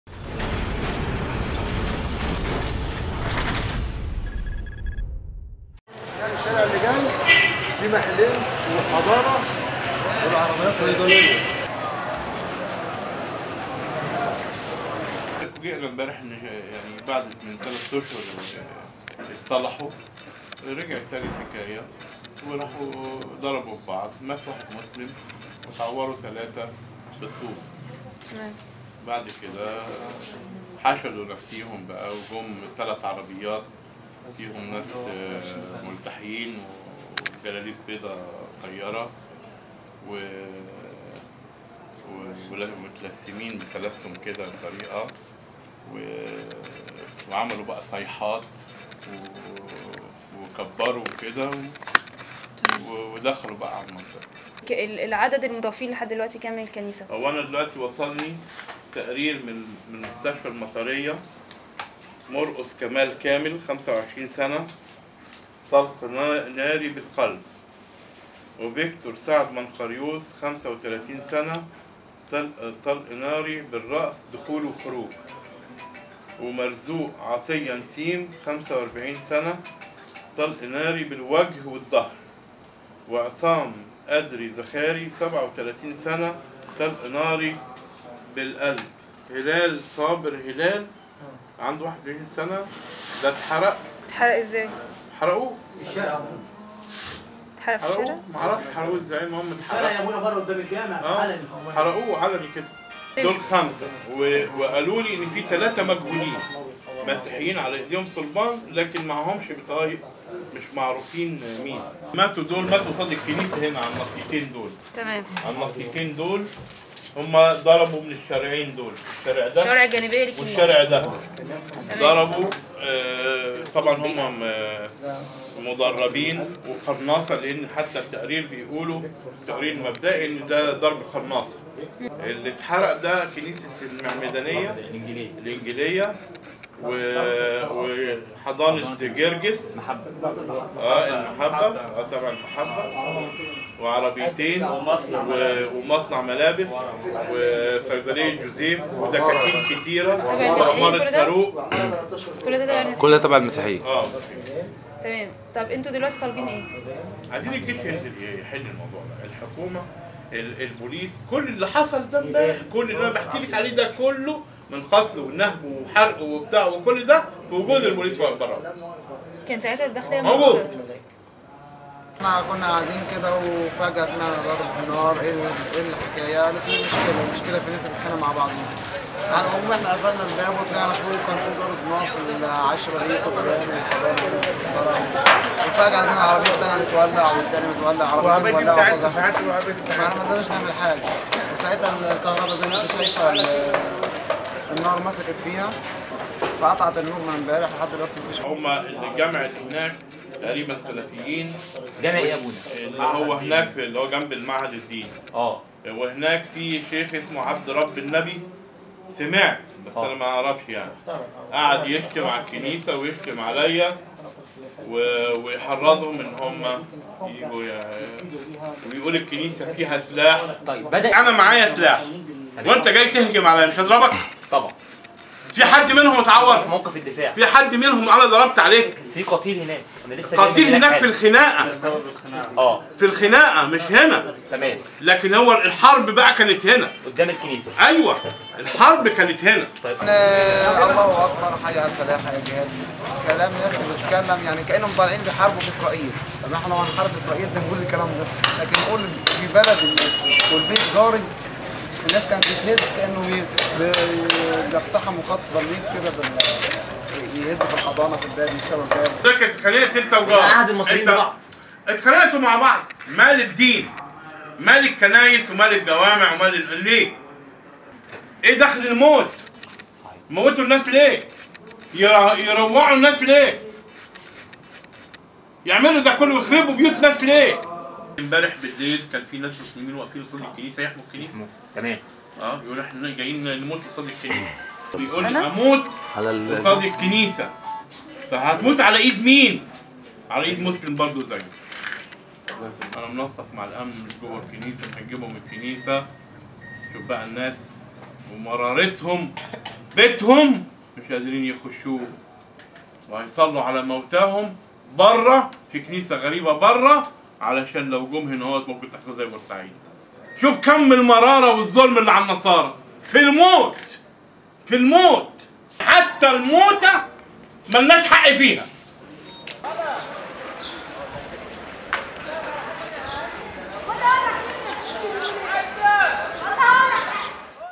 حقيقة الفتنة الطائفية بالخصوص
حقيقة الفتنة الطائفية بالخصوص راعي الكنسية يروي تفاصيل قتل 8 مسيحيين علي ايدي سلفيين وإحراق حضانة وبرج 13 طابق دون تدخل من الداخلية   تصوير محمد إبراهيم    مونتاج محمد أنور